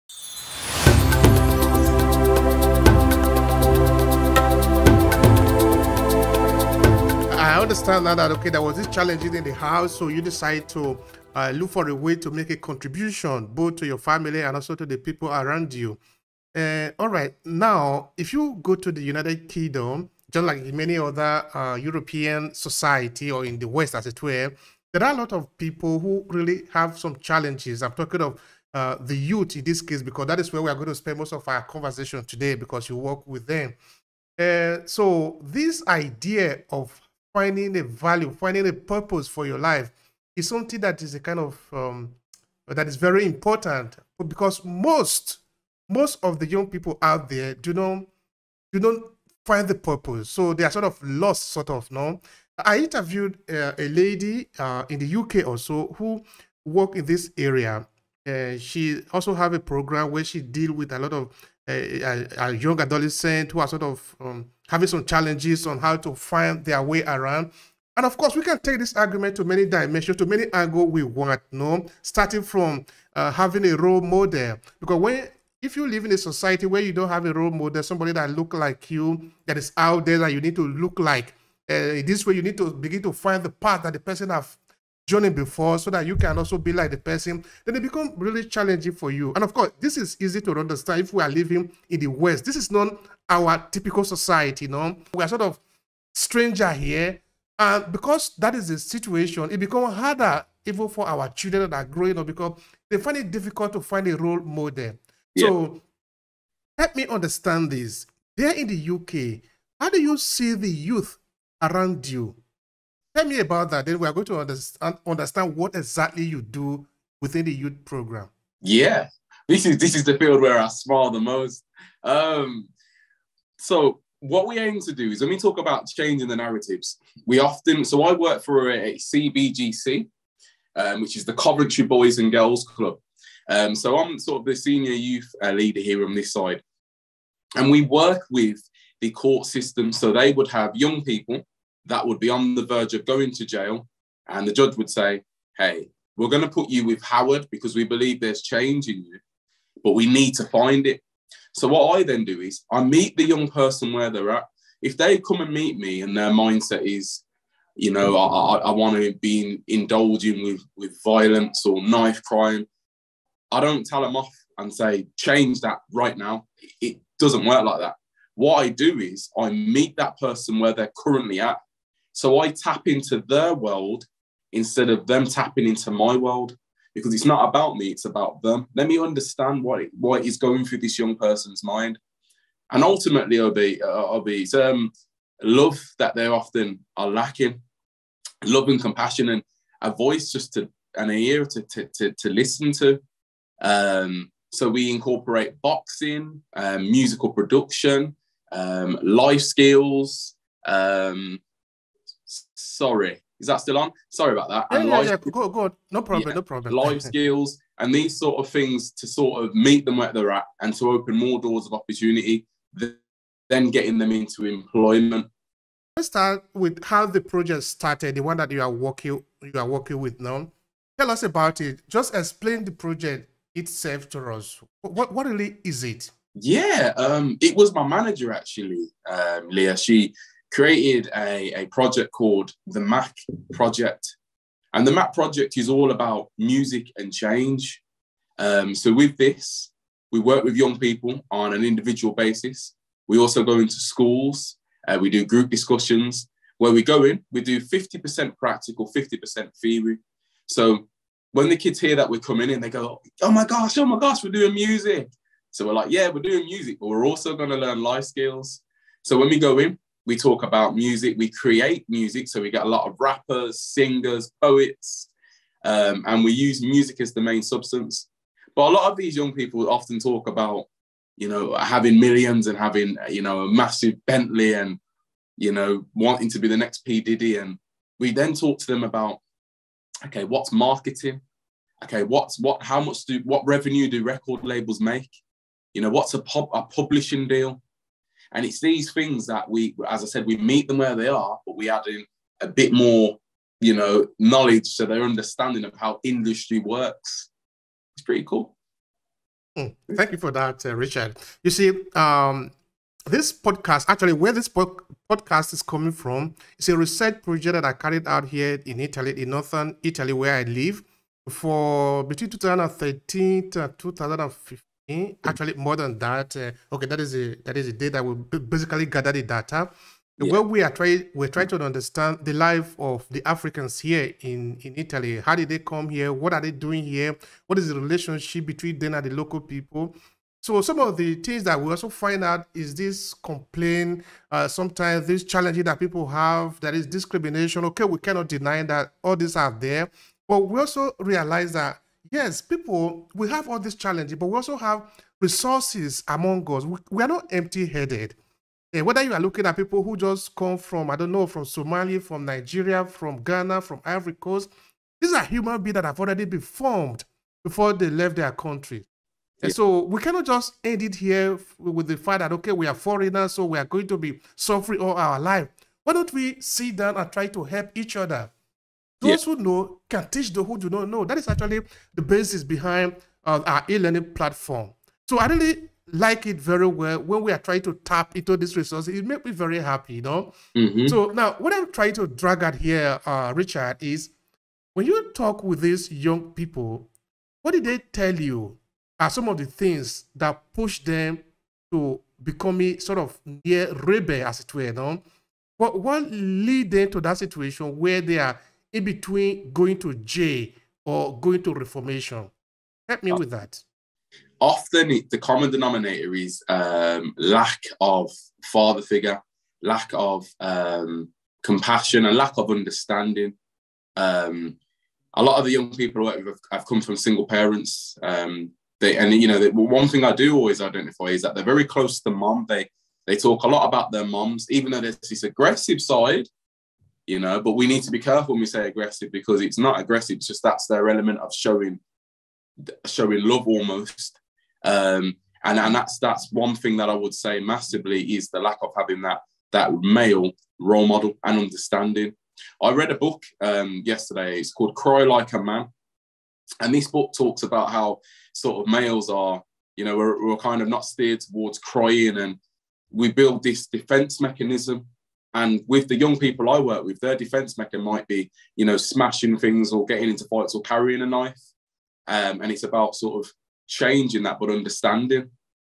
Changing the narrative - Senior Youth Worker, UK
Changing the narrative - Senior Youth Worker, UK ObehiPodcast. Howard Richards is a Senior Youth Worker and Project Coordinator (Coventry Boys & Girls Club), United Kingdom.

♥Useful information♥
- Love reading books, then join our New Digital Book Club

♥Resource links to get you started♥